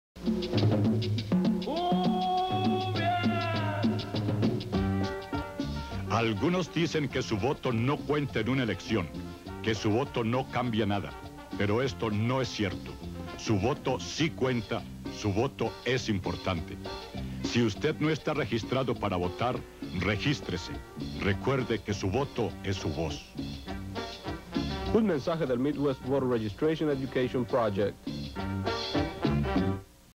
Midwest Voter Registration Education Project 1987 (Chicago)
From 1987, here is a PSA for the Midwest Voter Registration Education Project from Chicago.  Commercial en espanol.